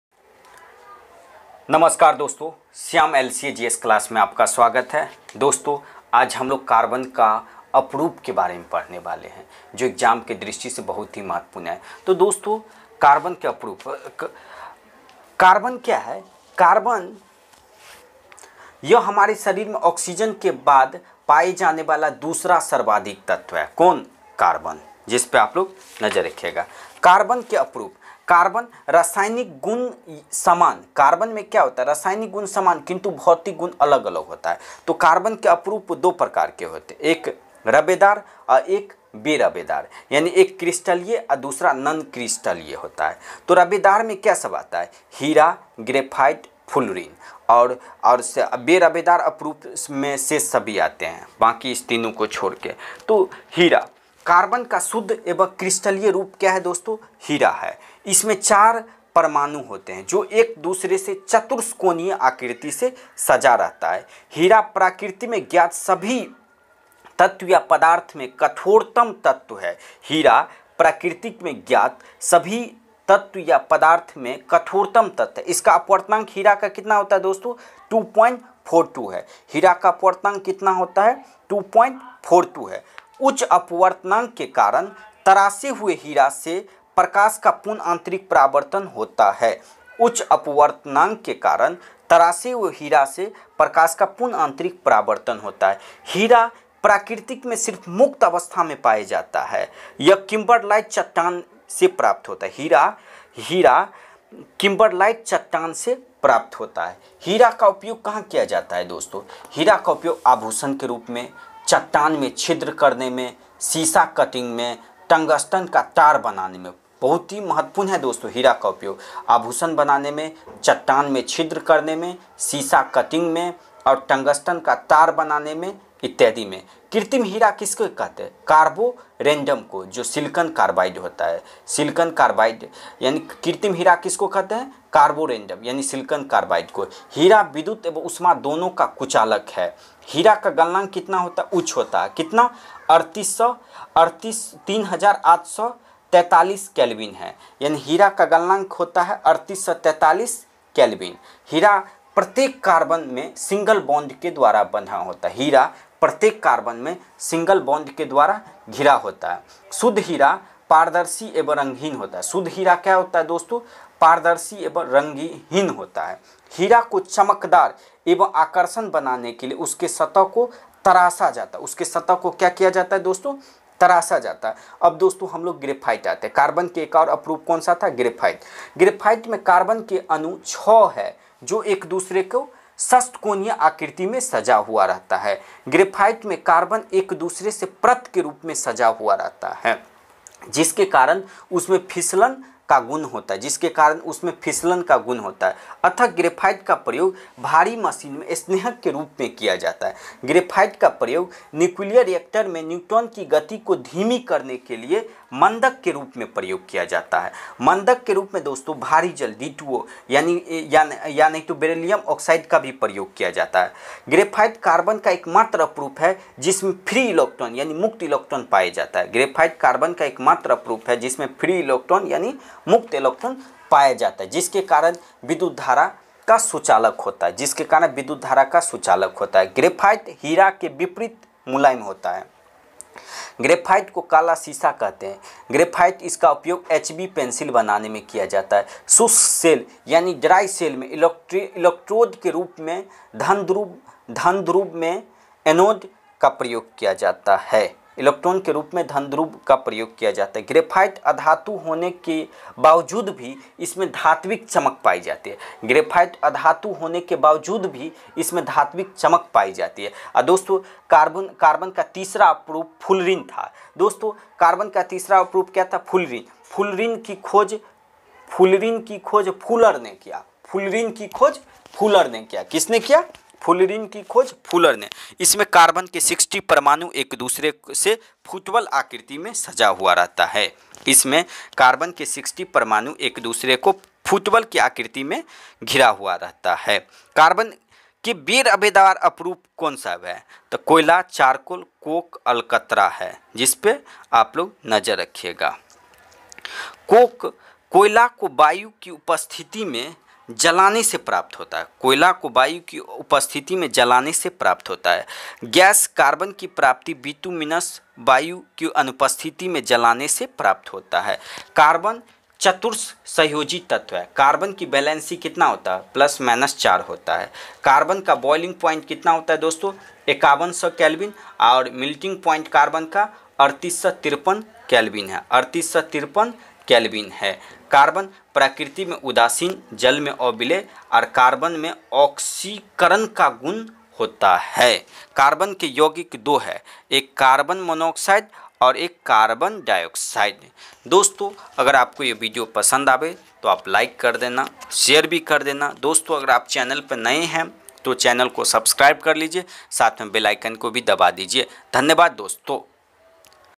कार्बन का अपरूप | Carbon ka aprup | Allotrope of carbon Chemistry Karbonn ka tin aprup Shyam lca
Shyam LCA GS MATH
कार्बन का अपरूप संबंधित महत्वपूर्ण प्रश्न
Allotrope of carbon
1 ful rin ki Khoj kisne kiya tha
2 ग्रेफाइट अधातु होने के बावजूद भी इसमें धात्विक चमक पाई जाती है
3 कार्बन चतुर्थ संयोजी तत्व है
4 कार्बन की बैलेंस इलेक्ट्रॉन  4
5 ग्रेफाइट को काला शीशा भी कहते है
6 ग्रेफाइट को काला शीशा भी कहते हैं